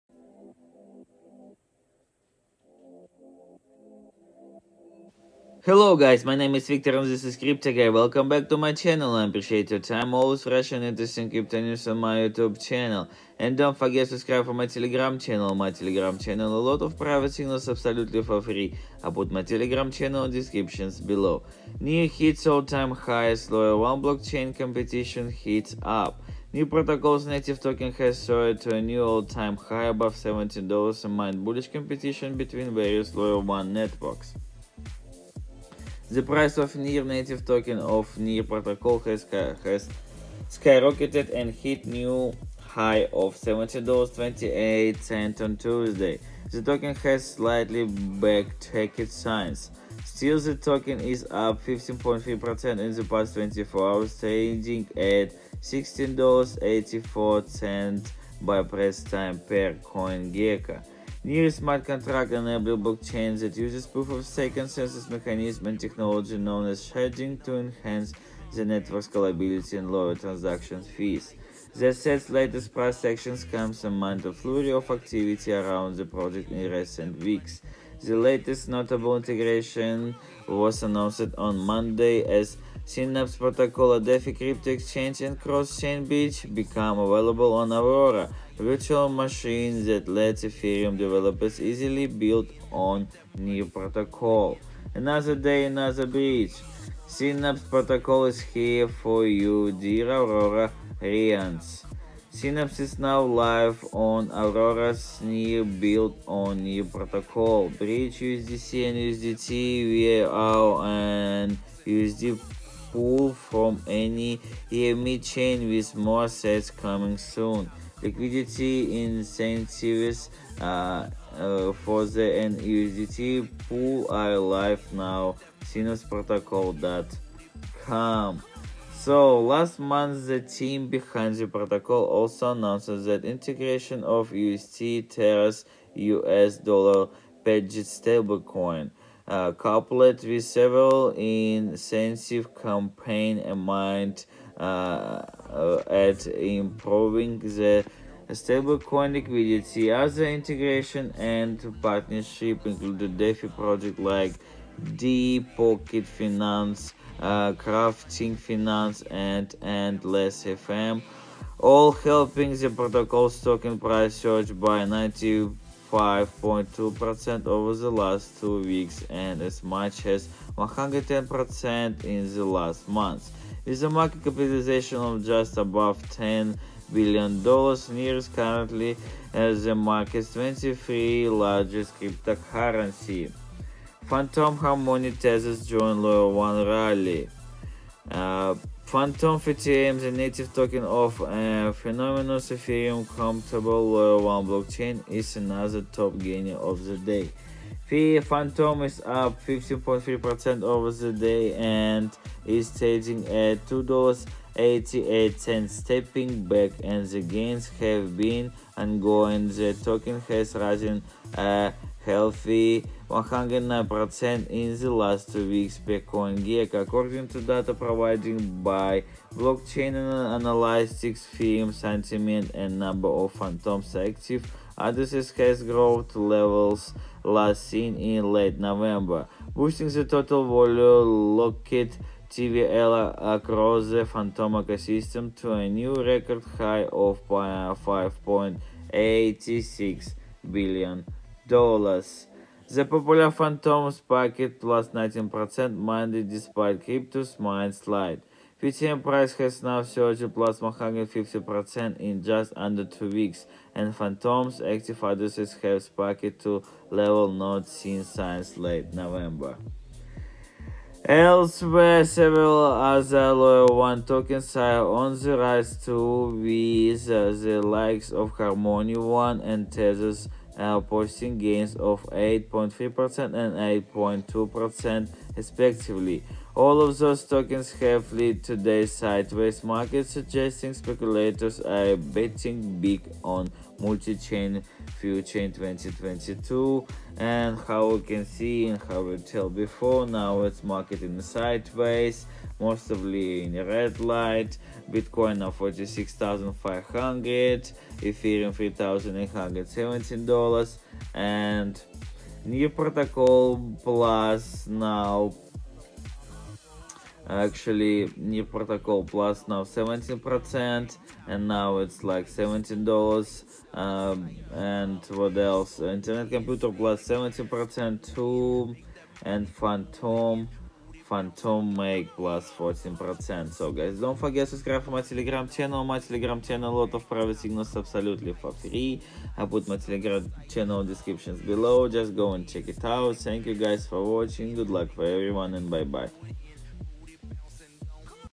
Near Hits All-Time High as Layer-1 Blockchain Competition Heats Up
Crypto News

Near Protocol’s native token has soared to a new all-time high above $17 amid bullish competition between various layer-1 networks.

The price of NEAR, the native token of Near Protocol, has skyrocketed and hit a new high of $17.28 on Tuesday.

The token has slightly backtracked since. Still, the token is up 15.3% in the past 24 hours, trading at $16.84 by press time, per CoinGecko.

NEAR is a smart contract-enabled blockchain that uses a proof-of-stake (PoS) consensus mechanism and a technology known as sharding to enhance the network's scalability and lower transaction fees.

The asset's latest price action comes amid a flurry of activity around the project in recent weeks.

The latest notable integration was announced on Monday as Synapse Protocol, a DeFi crypto exchange and cross-chain bridge, became available on Aurora, a virtual machine that lets Ethereum developers easily build on Near Protocol.

Last month, the team behind the protocol also announced the integration of UST, Terra's U.S. dollar-pegged stablecoin, coupled with several incentive campaigns aimed at improving the stablecoin's liquidity.

Other integrations and partnerships included DeFi projects like DePocketFinance, CraftingFinance, and EndlessFM, all helping the protocol's token price surge by 95.2% over the last two weeks and as much as 110% in the last month.

With a market capitalization of just above $10 billion, NEAR is currently the market's 23rd-largest cryptocurrency.